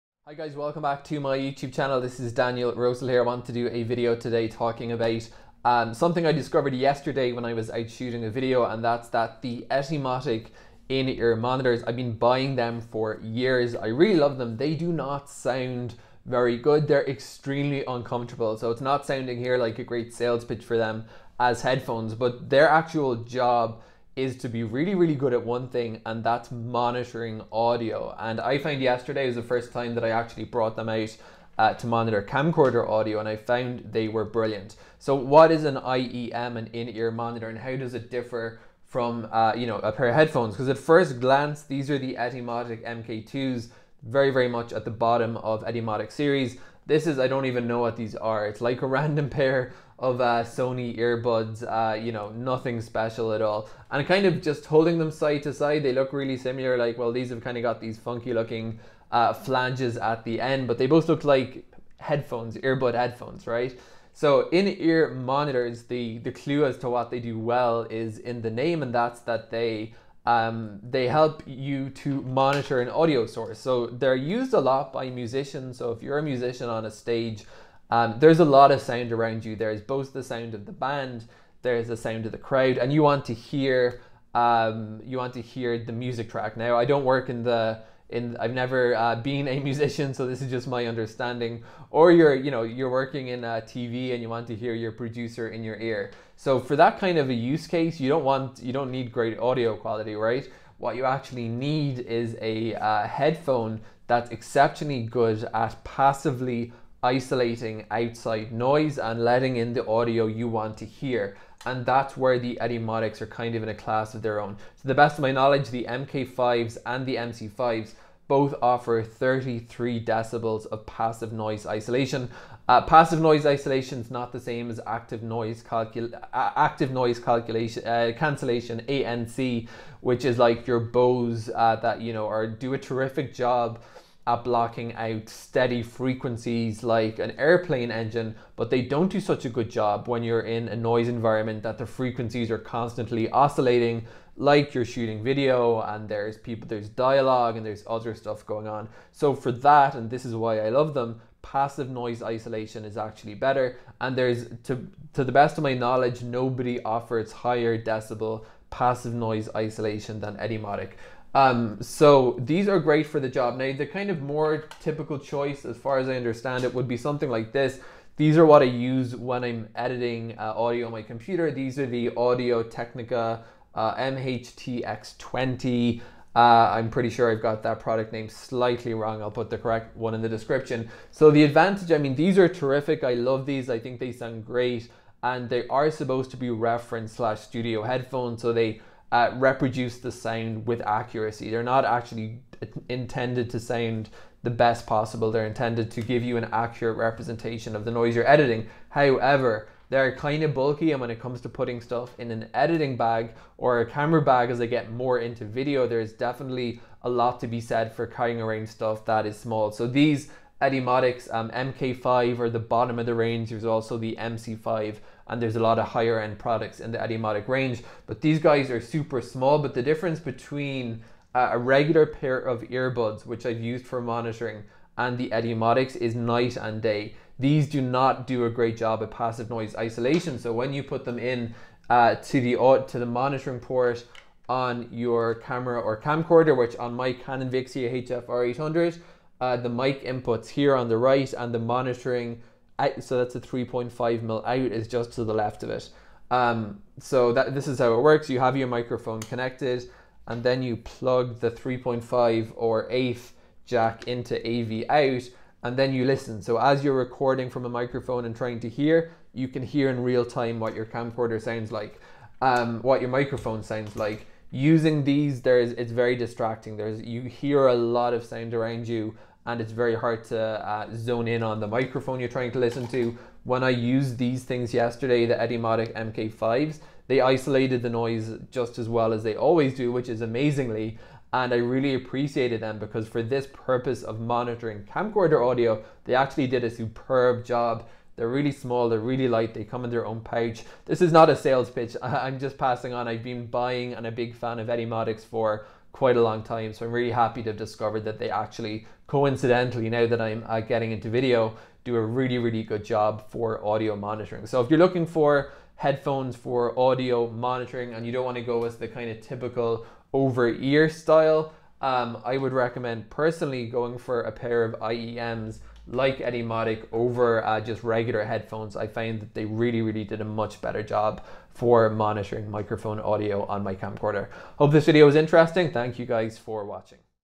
Etymotic IEMs: A Good Choice For Camera Audio Monitoring
Etymotic in ear monitors (IEMs) are good choices for monitoring camera audio - such as real-time monitoring of input sources such as microphones.